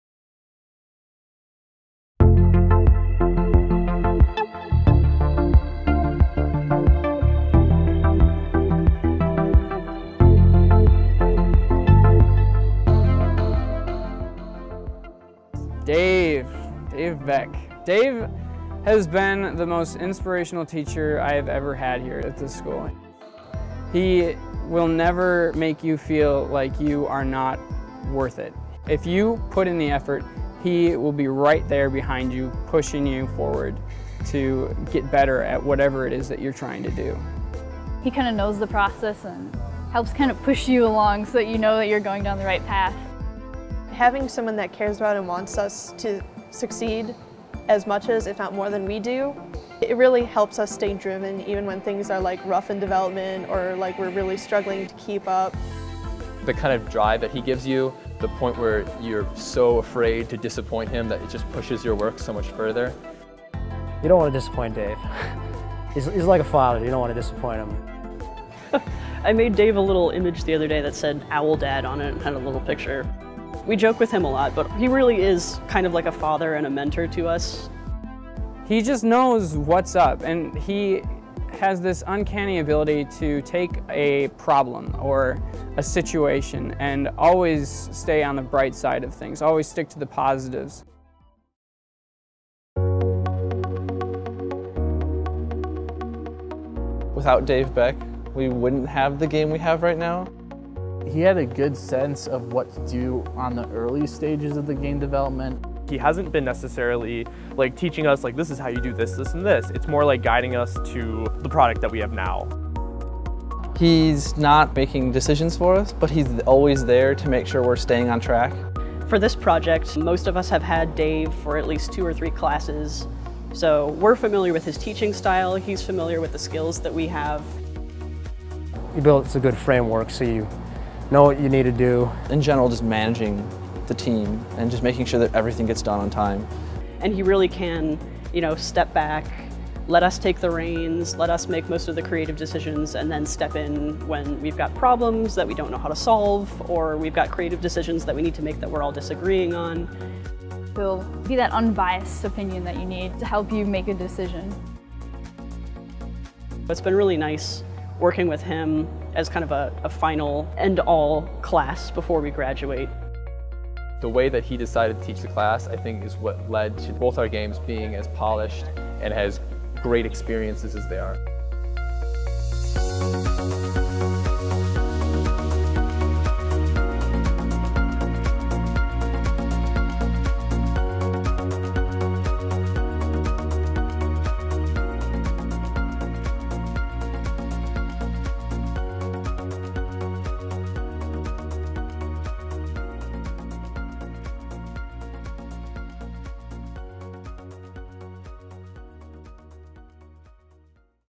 A UW-Stout Game Design Documentary: Behind the Games - Episode 7: Dave Beck
The students talk about how Dave Beck, their professor, gave them guidance throughout the capstone process. He was an inspiration, a mentor, sort of like a father figure, and he always pushed them to do their best, if not better.